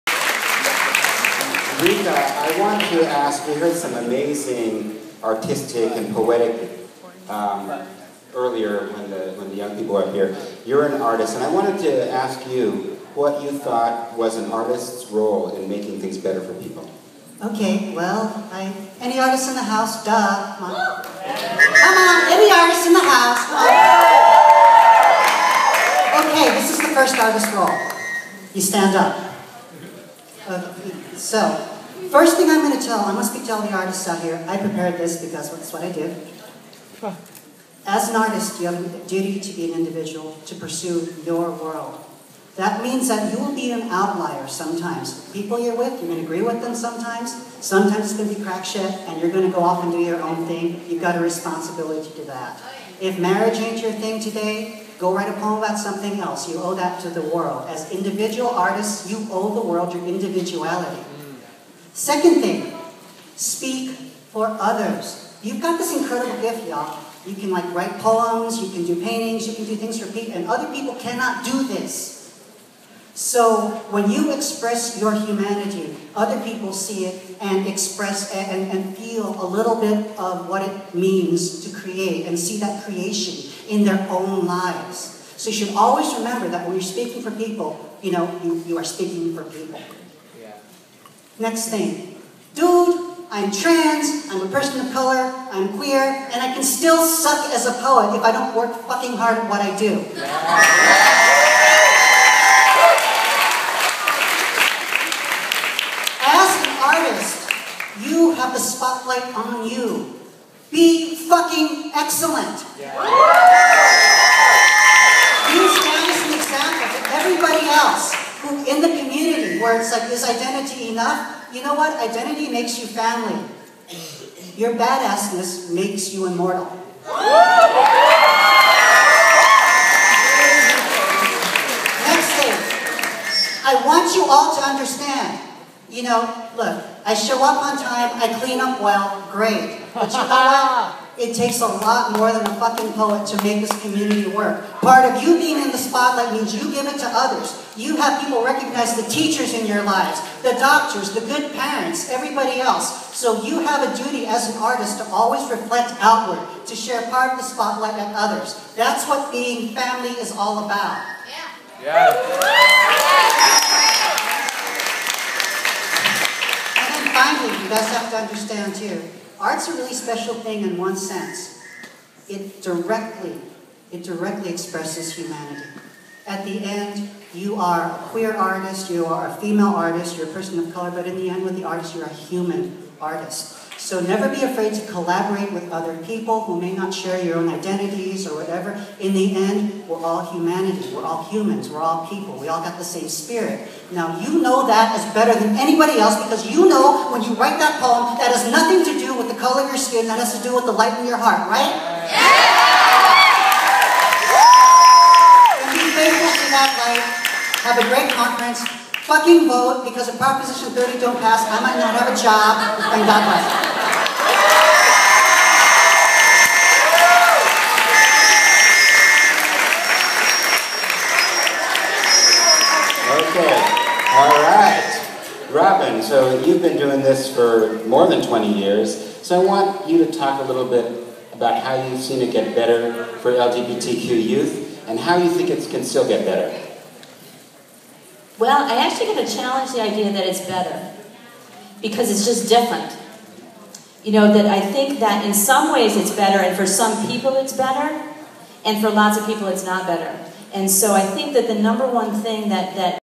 Ryka Aoki at Models of Pride 20
Ryka Aoki's Artist Manifest@ at MOP 20
with Dolores Huerta and Robin McHaelen
October 13, 2012